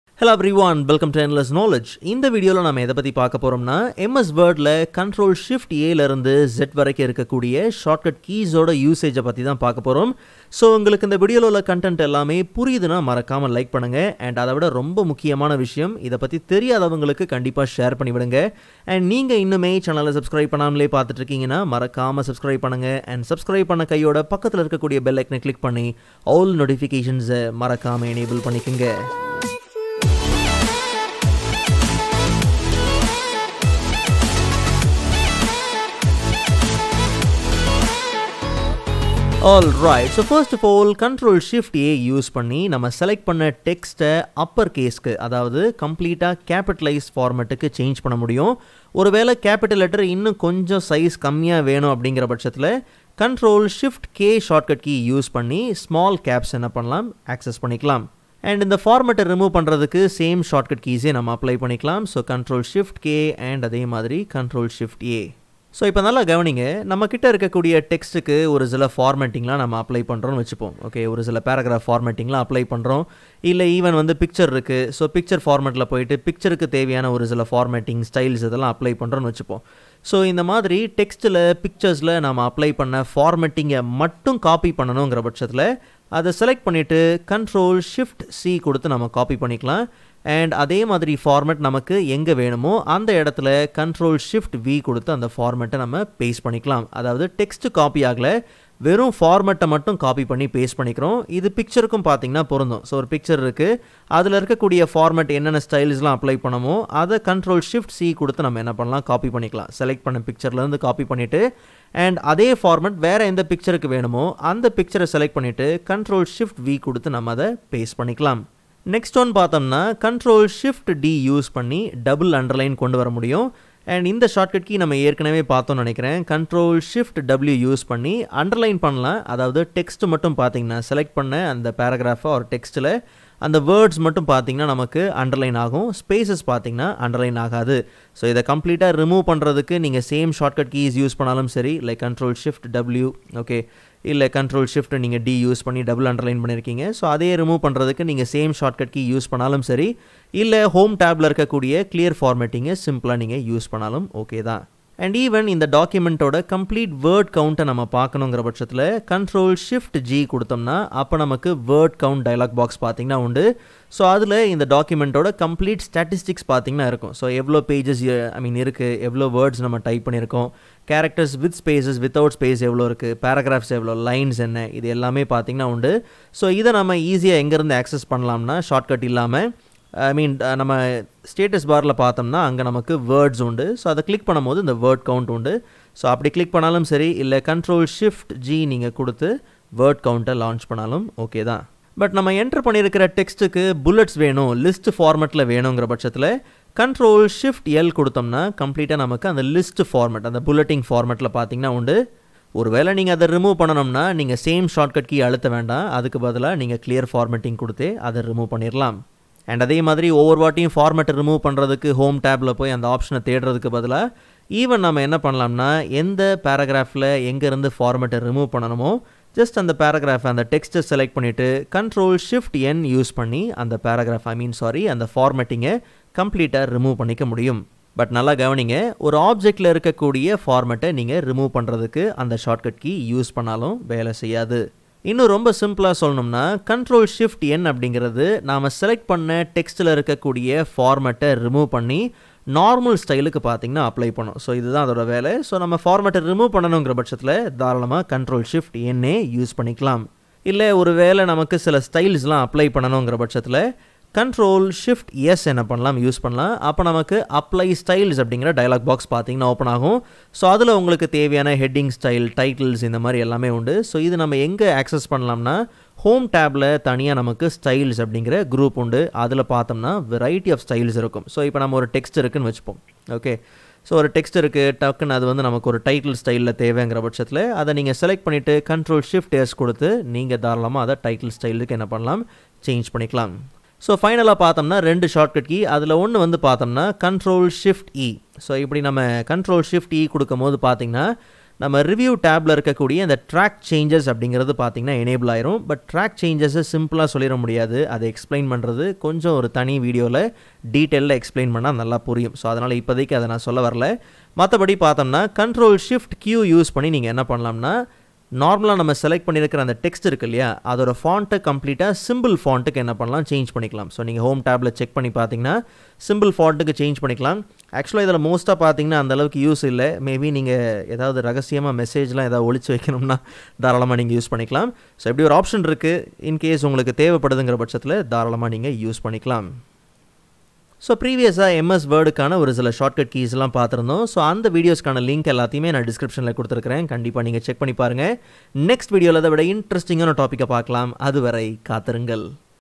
Ctrl + Shift + A to Z Shortcut keys in MS Word in Tamil
Ctrl + Shift + A to Z Shortcut keys step by step Tutorial.